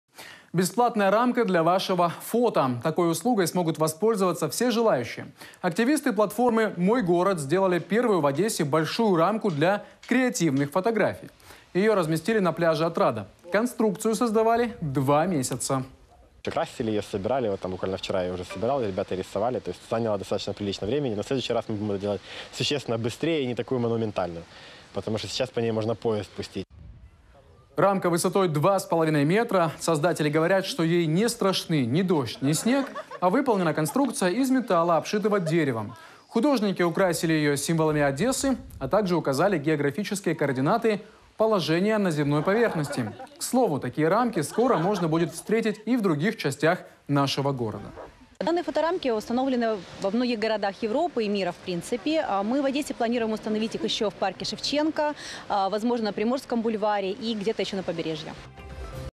Активисты платформы «Мой город» сделали первую в Одессе большую рамку для фото
Бесплатная рамка для вашего фото! Такой услугой смогут воспользоваться все желающие. Активисты платформы «Мой город» сделали первую в Одессе большую рамку для креативных фотографий. Ее разместили на пляже «Отрада».

Конструкцию создавали два месяца. Высота рамки - два с половиной метра. Создатели говорят, что ей не страшны дождь и снег. Выполнена конструкция из метала, обшитого деревом. Художники украсили ее символами Одессы, а также указали географические координаты положения на земной поверхности. К слову, такие рамки скоро можно будет встретить и в других районах города.